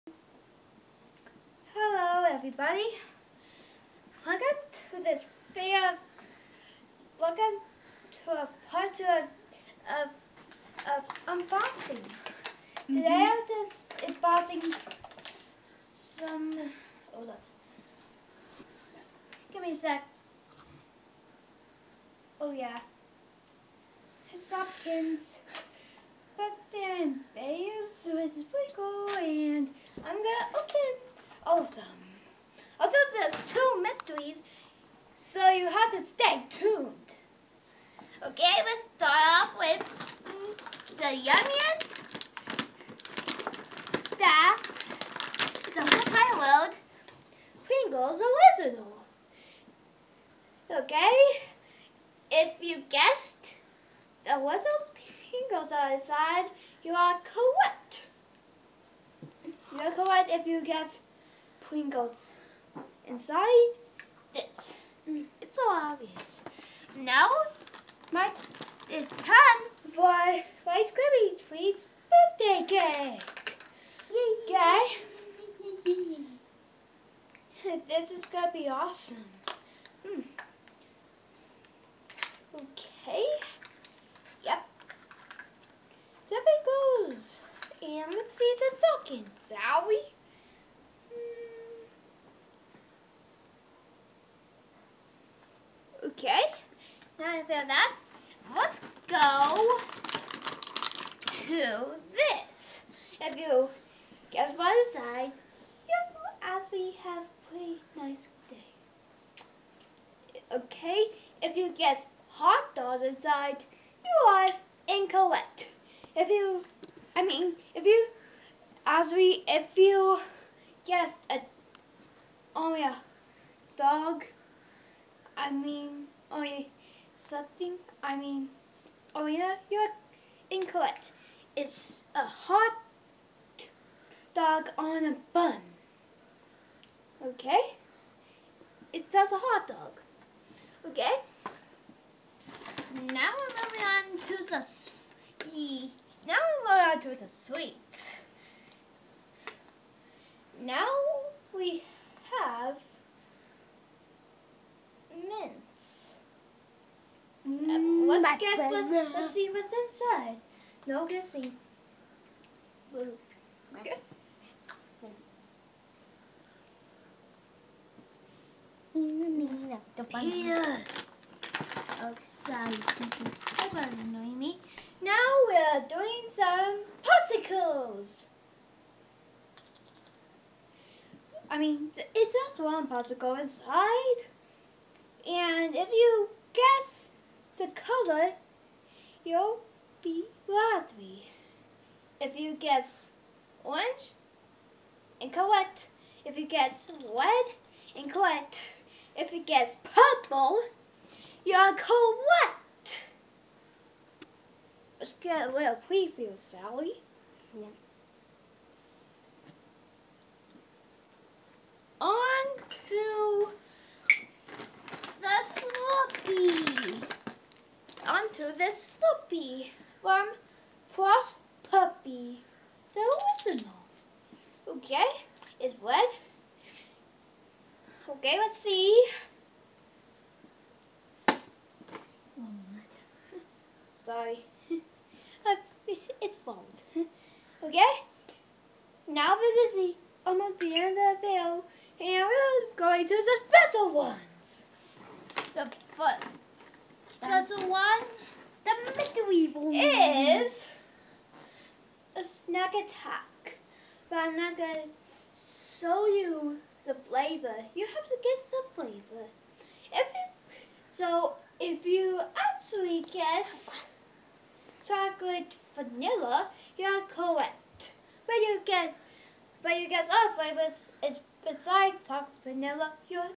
B & P's Shopkins Real Littles Reveal - Part 1
B & P share the Shopkins Real Littles they got in their Mega Pack.